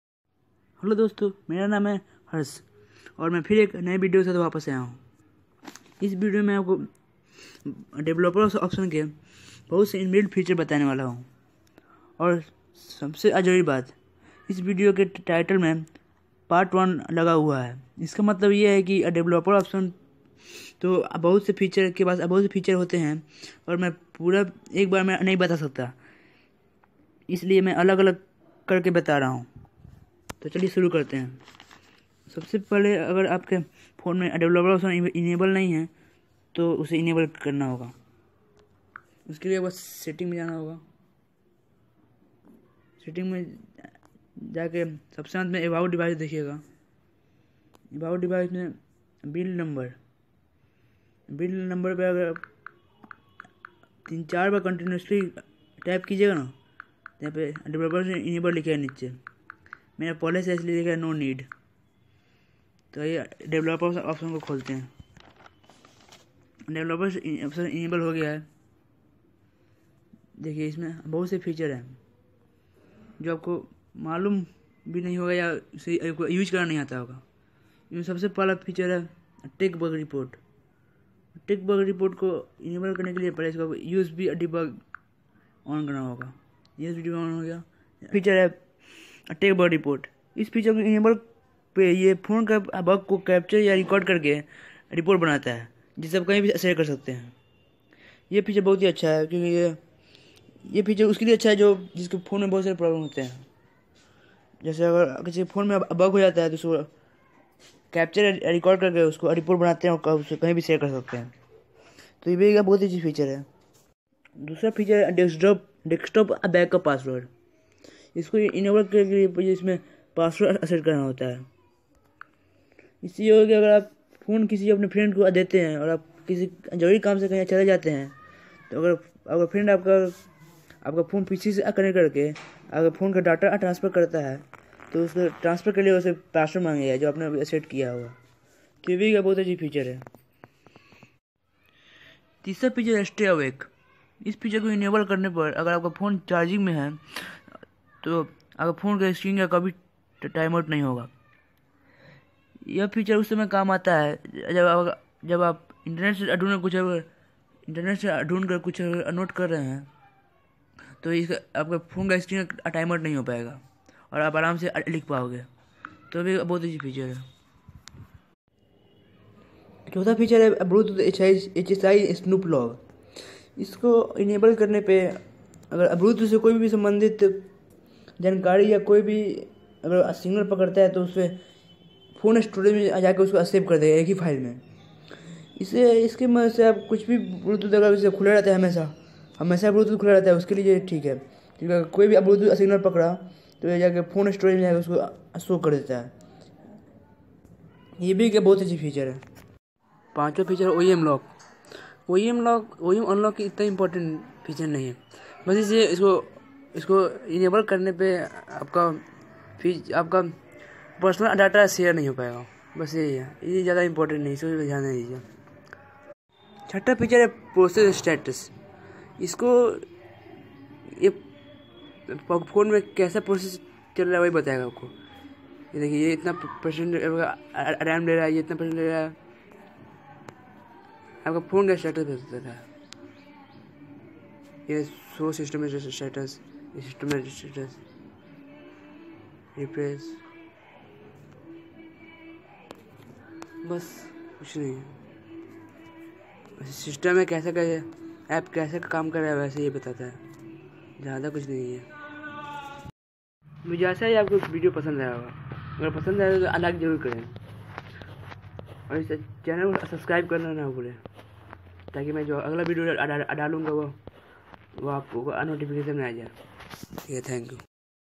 Description of all the interesting feature of Developer Option .....!! PART 1
Description of all the interesting feature of Developer Option .!!
PART 1
Explain In Hindi Series is Very Popular.
I will upload PART2 in next video